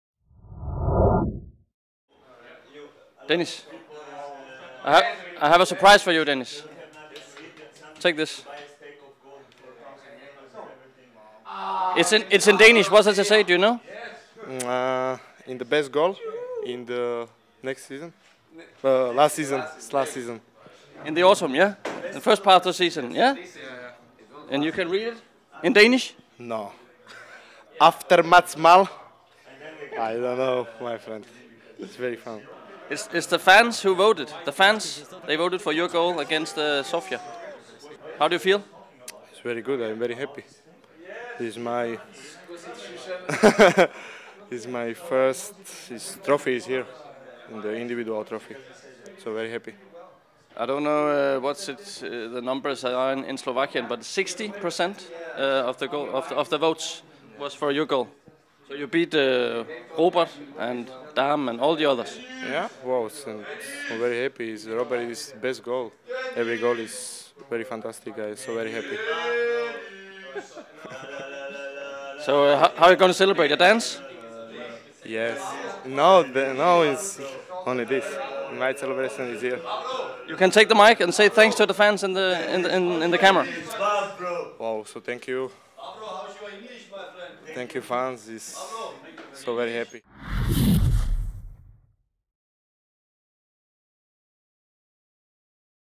Vavro vandt afstemningen om Efterårets Mål: Jeg er meget glad!
Denis Vavro blev en sikker vinder i fan-afstemningen om Efterårets Mål. FCK TV overrakte ham æren på årets første træningsdag. Se og hør hans takketale her!

profilnavn: fc_kobenhavn